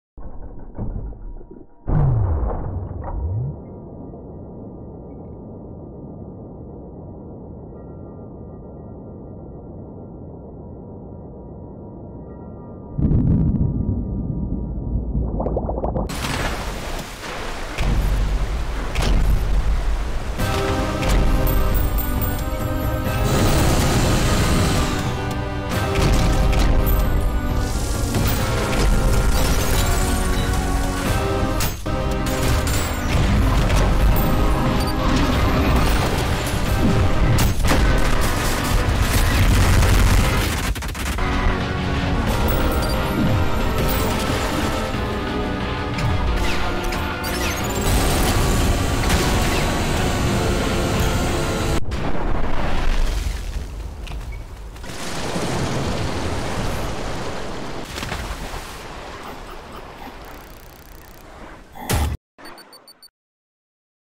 So, you're struggling to beat Leviathan?
I heard you were struggling to kill leviathan, I'm always here to help.
(More to this series coming soon)